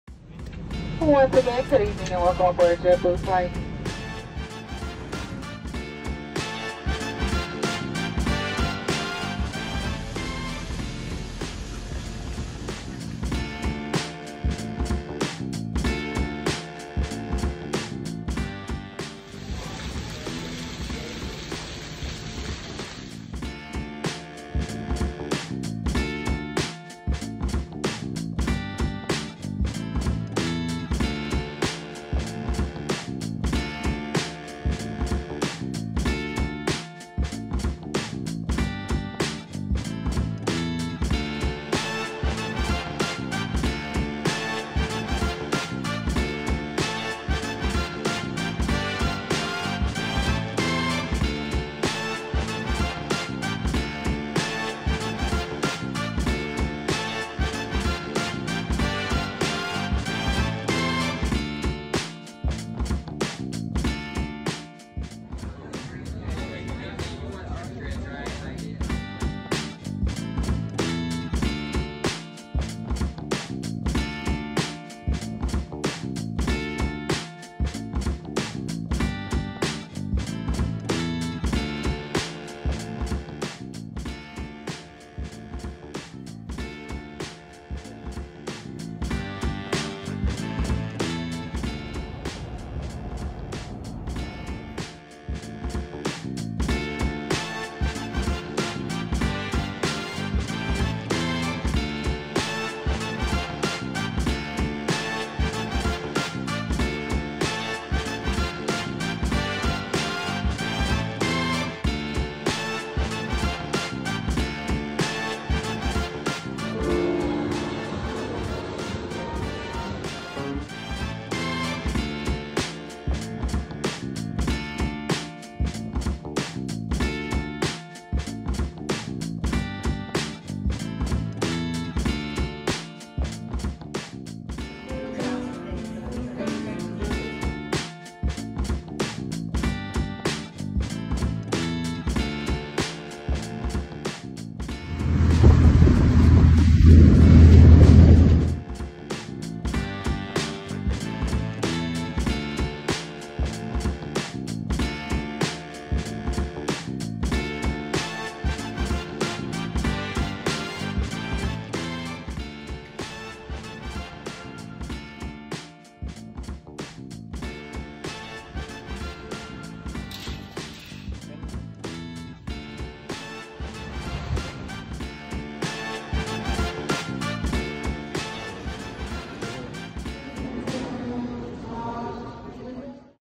Our Trip to Savannah, Georgia ( JFK to SAV)
We took a weekend trip to Savannah, GA! First time exploring this beautiful historic city and we had the best experience! We stayed at the Westin Resort and Spa Hotel! We took the Savannah Bella Ferry to the downtown district! We visited the Squares, we had dinner with “Ghosts”. Checked out the JW Marriott at the Plant Riverside District and visited the design district. Oh and not to forget all the amazing establishments such as 17hundred90 restaurant, Leopold’s Ice Cream, and so much more!